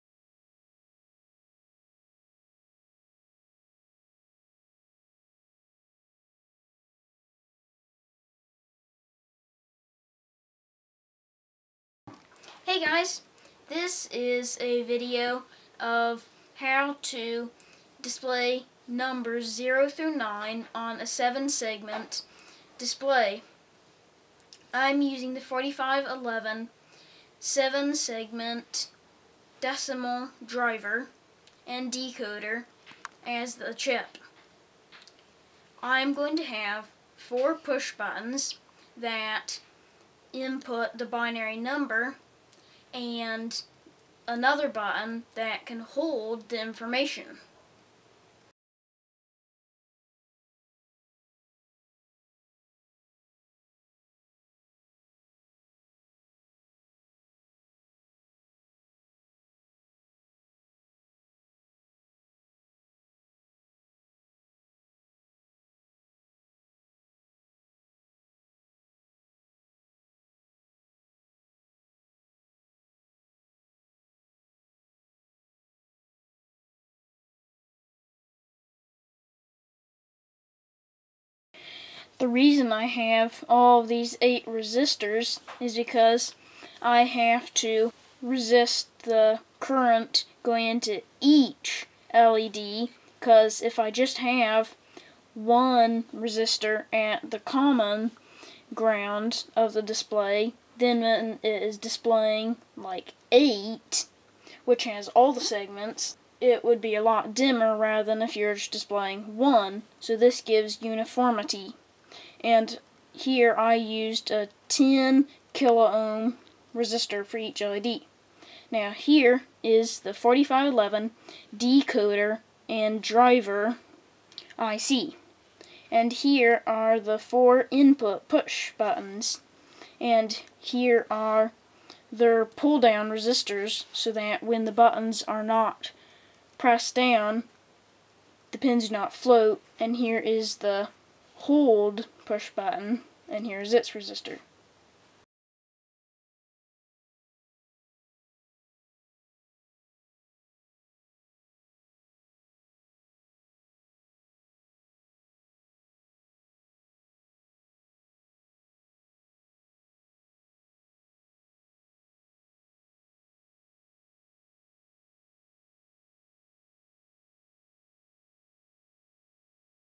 A 0 to 9 binary to seven segment display converter using the 4511 IC and the Seven segment 5161AS
A seven segment display converter circuit using the 4511 IC.